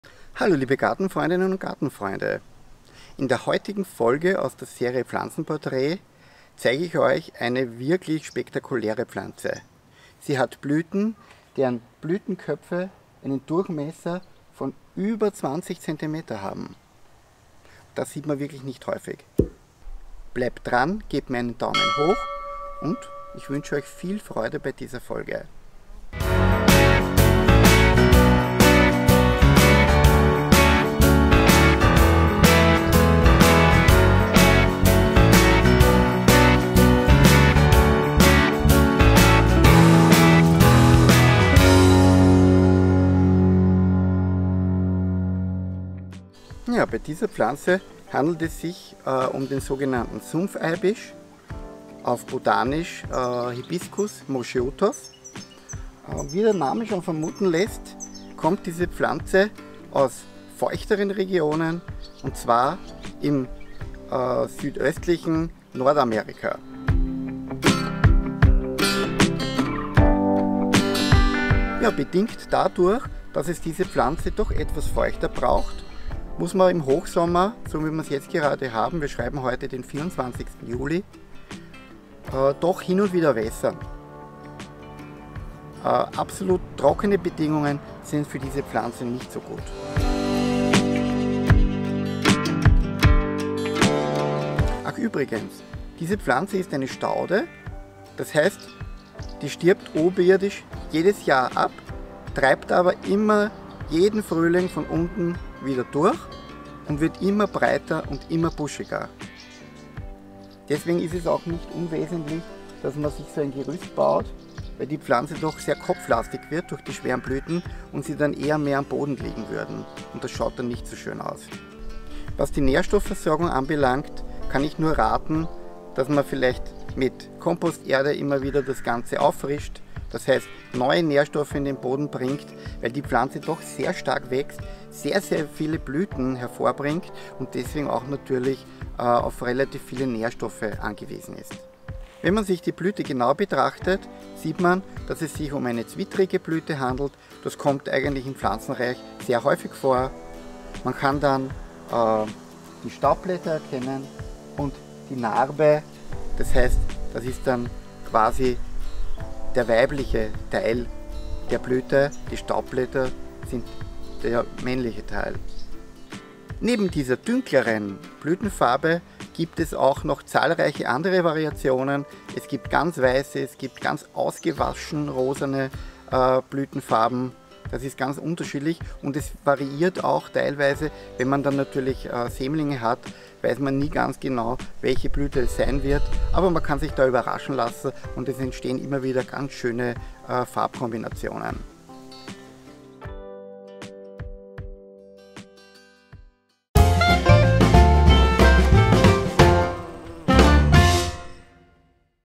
Pflanzenportrait - Sumpfeibisch
Heute stelle ich Euch wieder einmal eine Pflanze mit einer unglaublich spektakulären Blüte vor.
Es handelt sich um den Sumpfeibisch oder auch Sumpfhibiskus. Botanisch trägt diese Pflanze den Namen Hibiscus moscheutos und wie es der Name schon verrät, benötigt es diese Staude im Sommer etwas feuchter. Die Blüten können übrigens Blütendurchmesser von mehr als 20cm bekommen.
In diesem Sinne kann ich Euch aus eigener Erfahrung diese Pflanze für Eure Gärten absolut empfehlen :-)

Schreibt mir doch bitte auch etwas in Kommentare. Darüber würde ich mich sehr freuen :-)

Euer Andreas aus dem Garten Steinfeld